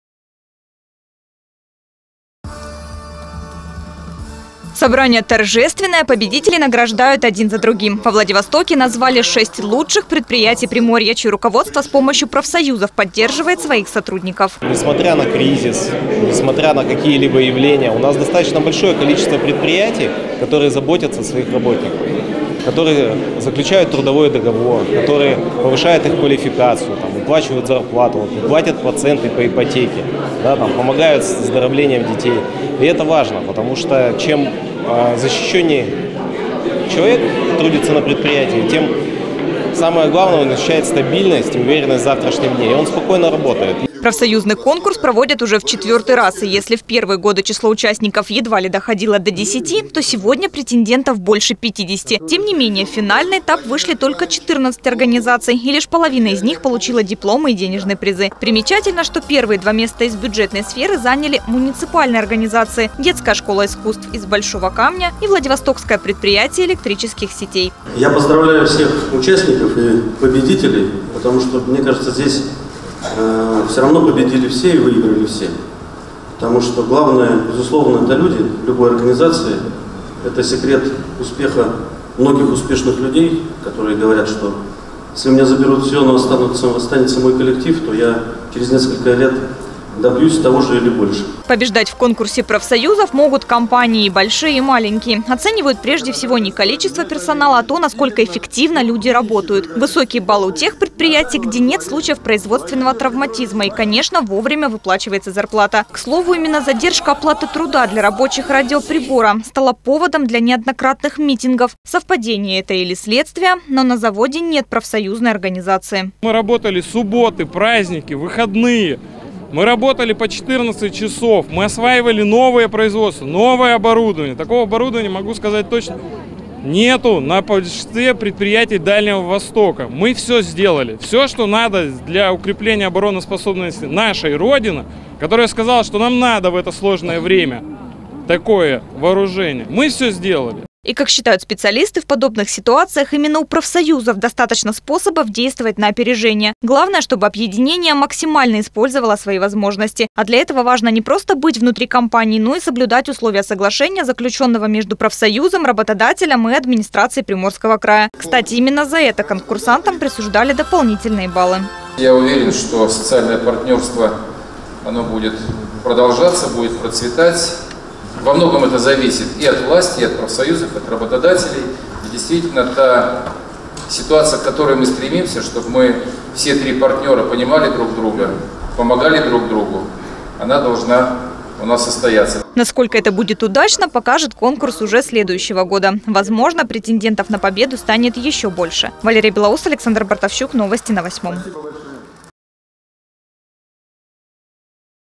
Что составляет самый большой капитал в компании?
Главный капитал для предприятия – это люди, считают победители конкурса «Социальная поддержка». Во Владивостоке наградили лучшие профсоюзы края.